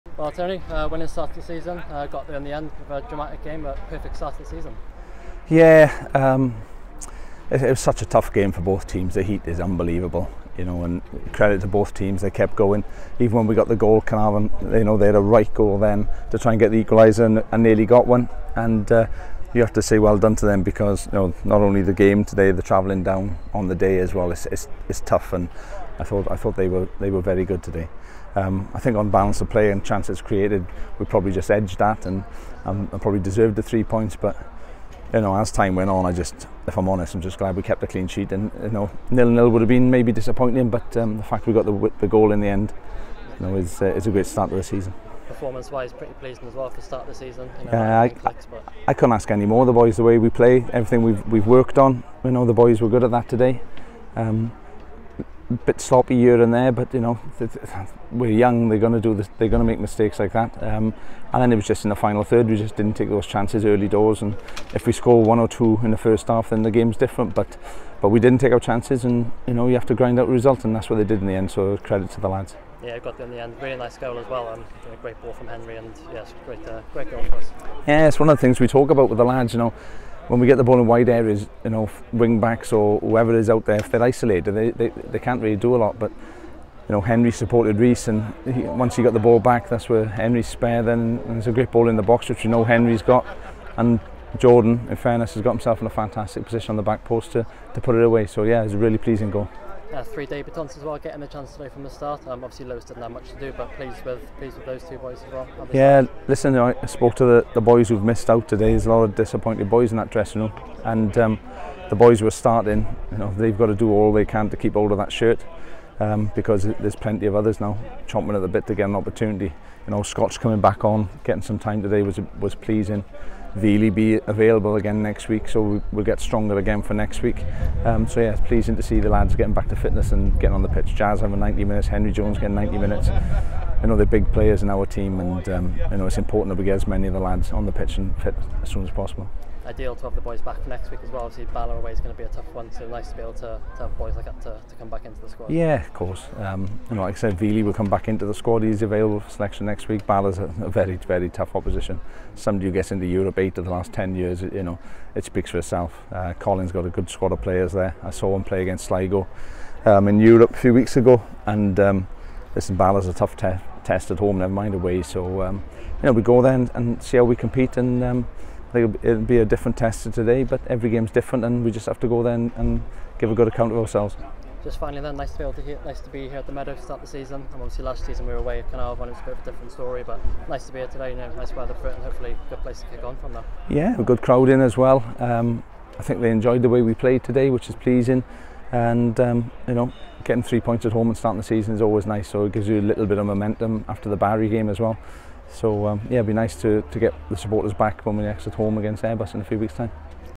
INTERVIEW | Tony Pennock on victory over Caernarfon Town | JD Cymru Premier
Manager Tony Pennock reflects on the 1-0 victory over Caernarfon Town on the opening day of the 2022-23 JD Cymru Premier season.